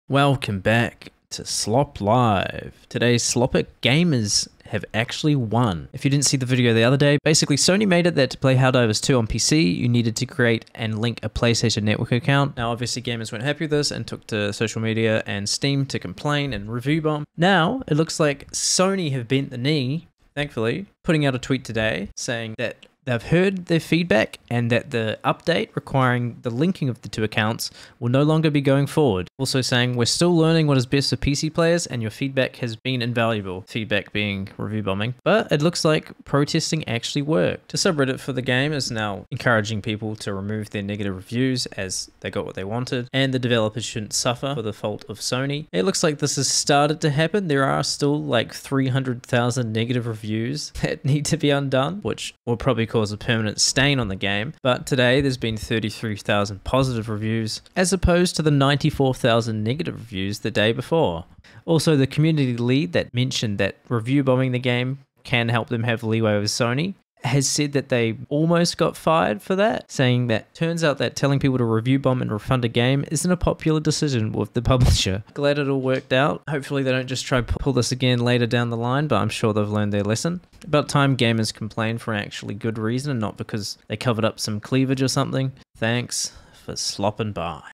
Helldivers 2 Situation is Actually Fixed
sony walked back their helldivers 2 decision, a win for democracy